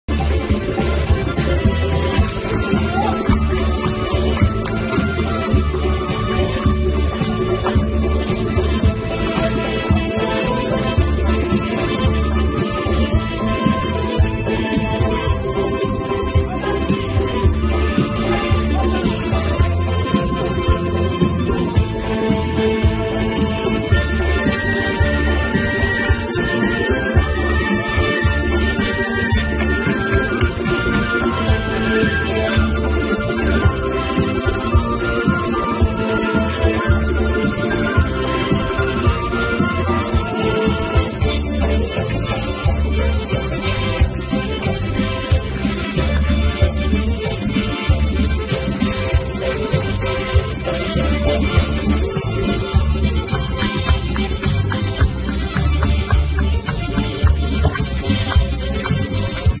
Georgian Folk Dance Ensemble "Khorumi" in Copenhagen [ DR Konserthuset ]
Rehearsal before celebration of GE Independence & 25th anniversary of diplomatic relations between Georgia & Denmark in Copenhagen DR Konserthuset